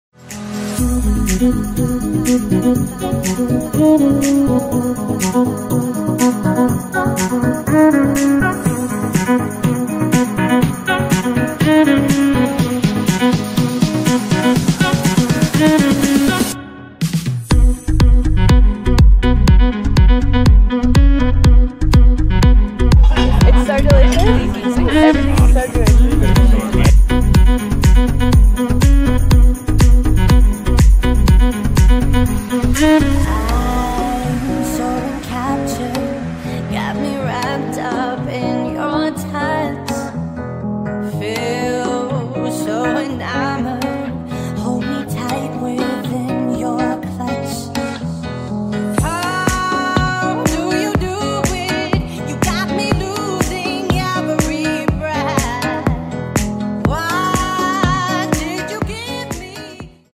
Swisse Winter Wellness Retreat | Nutrition, Naturopathy & HIIT
Highlights from the incredible @SwisseAU Winter Wellness Retreat ✨ We said sayonara to winter colds 👊 by smashing a workout with @mrtimrobards, a nourishing lunch by @gcalombaris, a cooking class with dietitian @simone_austin, and being treated to Swisse facials and a NatChat with a Swisse naturopath 🌿
The lunch table was my Pinterest board come to life @tussie_mussie_vineyard_retreat